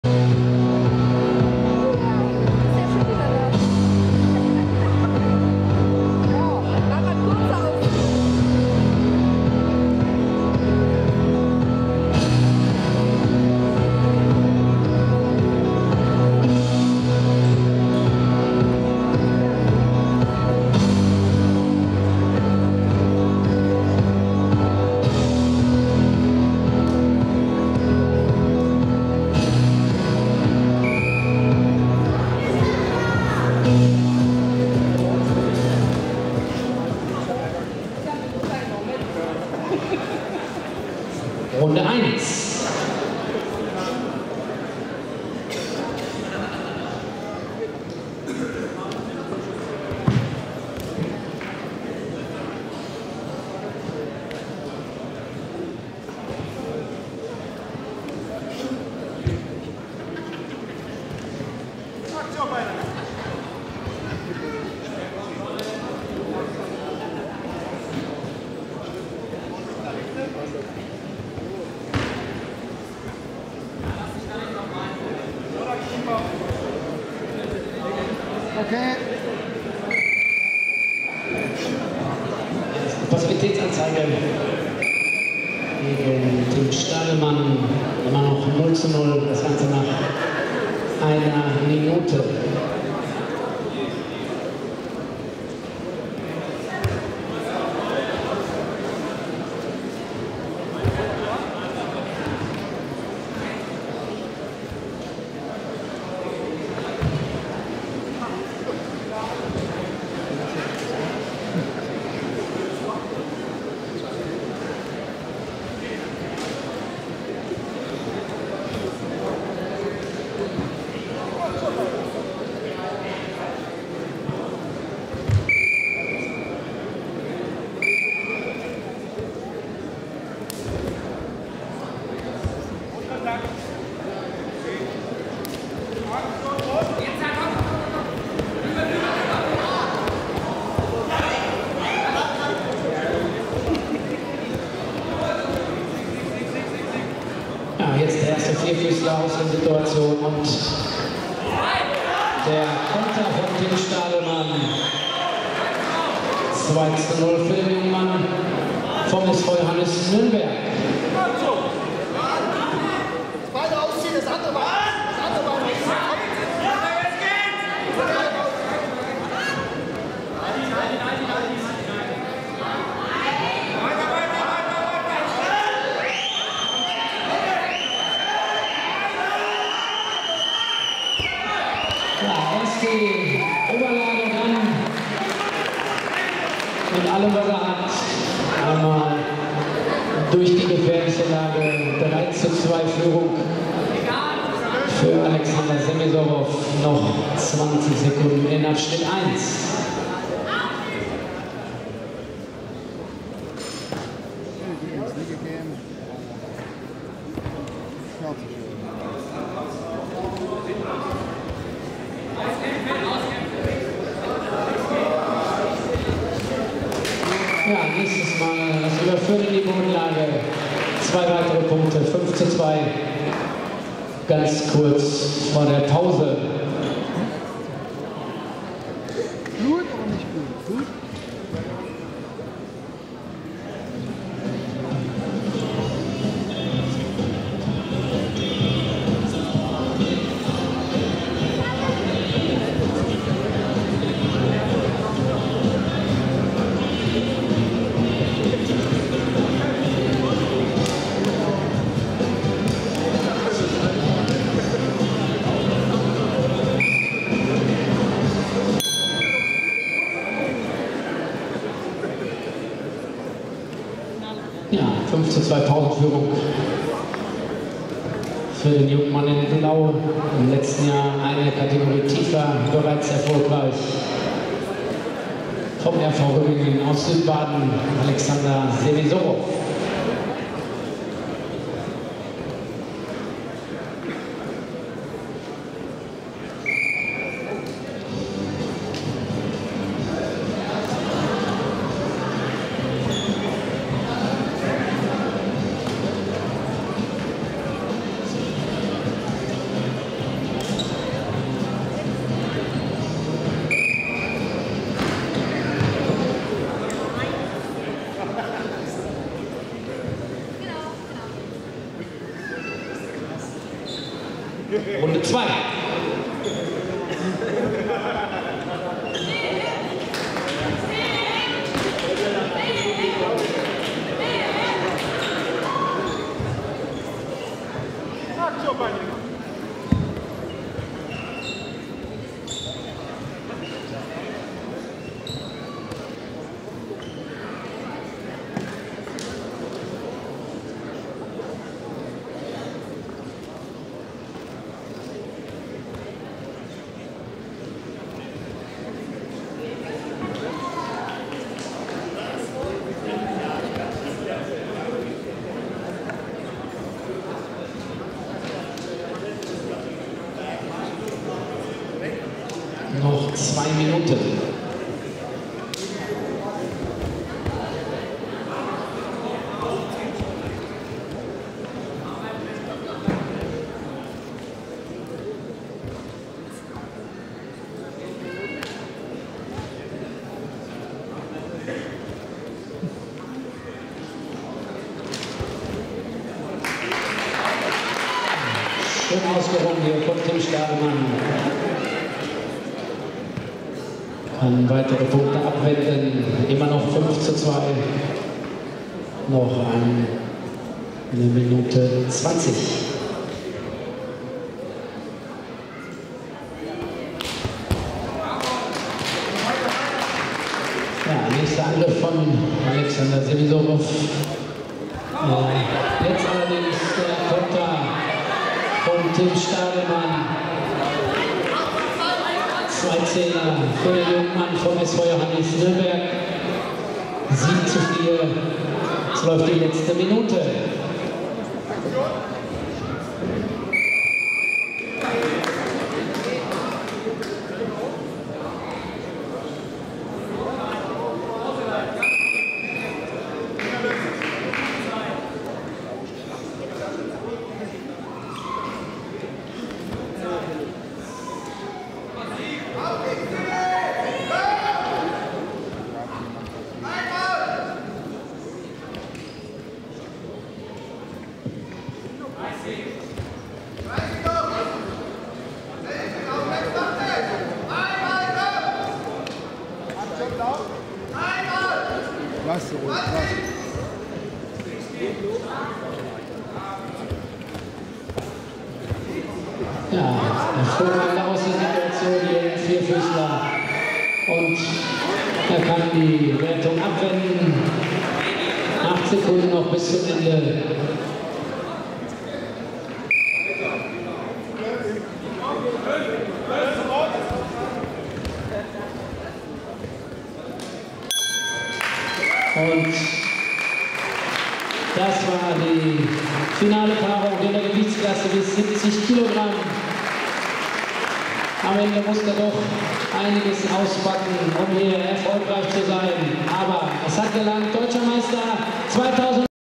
Deutsche Meisterschaften Männer Freistil - 70kg Finale Tim Stadelmann vs. Alexander Semisorow
RINGEN - Deutsche Meisterschaften Männer Freistil - 70kg Finale Tim Stadelmann vs. Alexander Semisorow